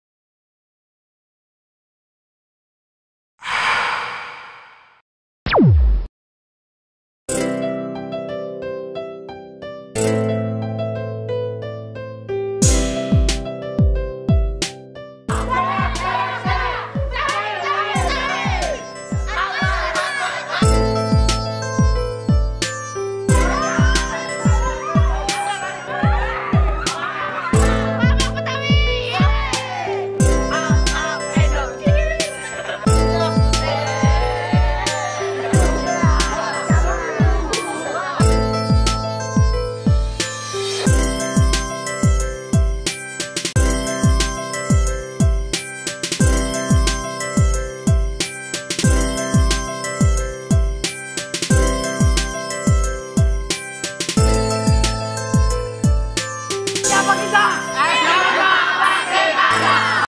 MPLS #2 Liputan Bakti Karya (Kelas Multikultural)
Sekilas kegiatan hari ketiga MPLS di SMK Bakti Karya Parigi Pangandaran. Kali ini, Kang Ai sebagai pengisi materi tentang "Merayakan Kebhinekaan Indonesia" pada sesi Teachers Day.

Siswa tampak asyik bermain sambil pengenalan konsep utama  Kelas Multikultural.